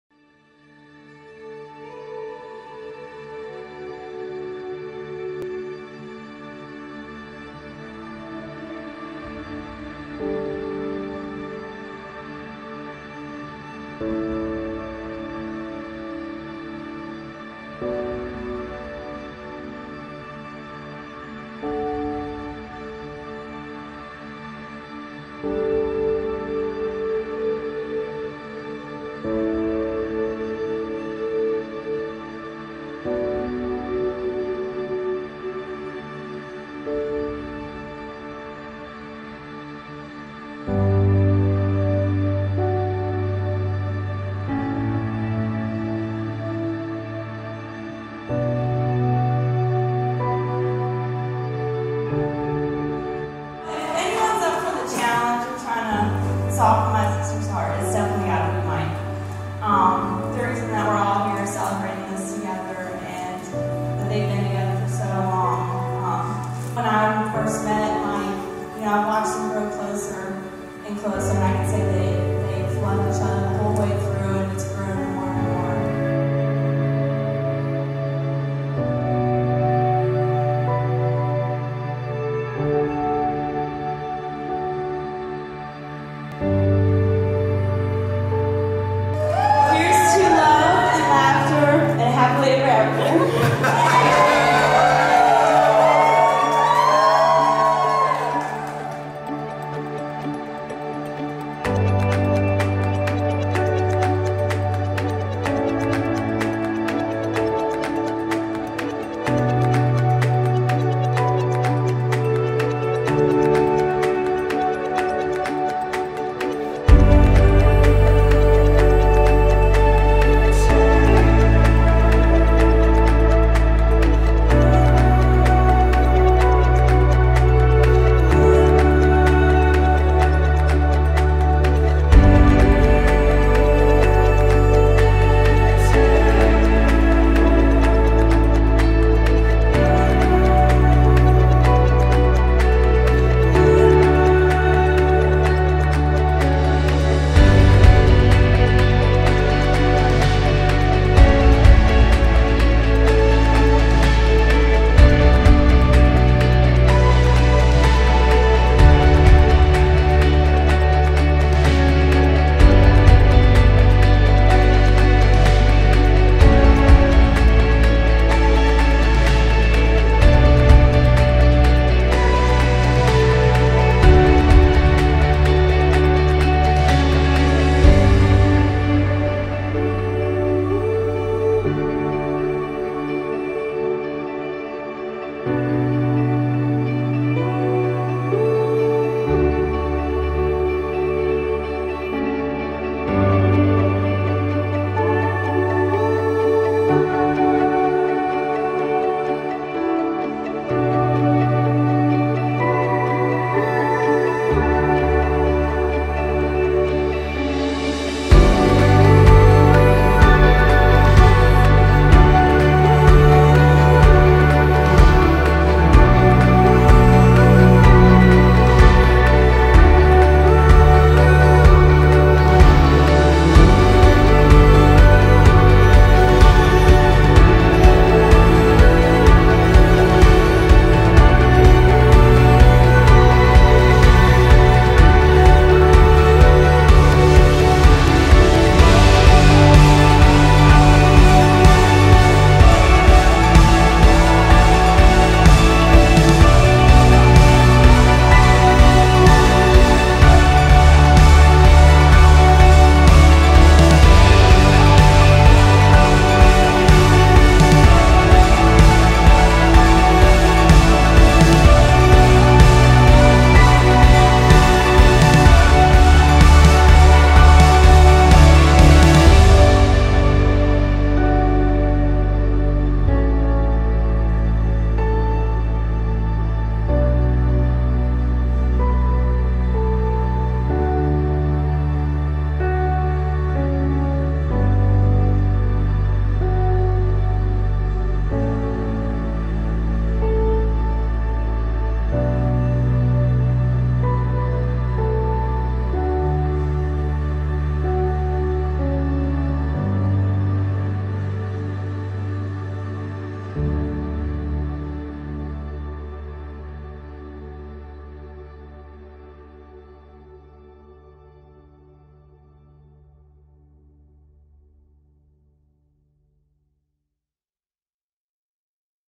Michael and Hayley
We joined Michael and Hayley for their wedding day at The Barn at Timber Creek. We are glad we were able to join them and capture their special day!